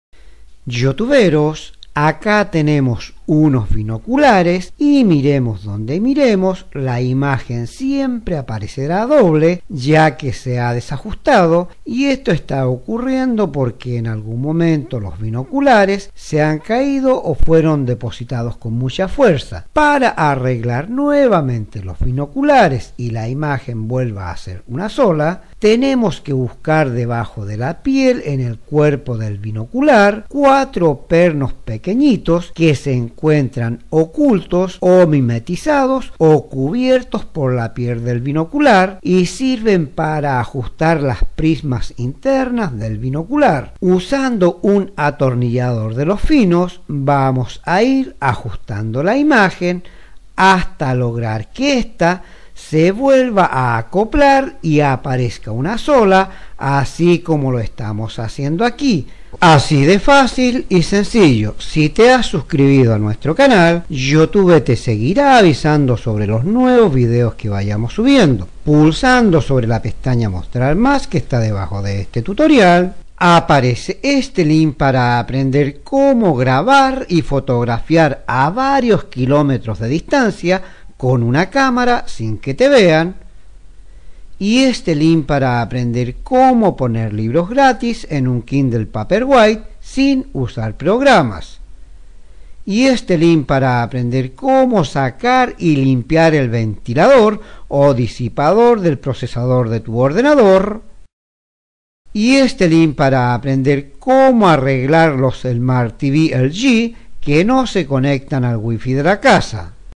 Reparar Prismáticos que se ven dos imágenes - fácil de hacer
Reparar Prismáticos que se ven dos imágenes, ya que se han desajustado por alguna caida o golpe es muy fácil de reparar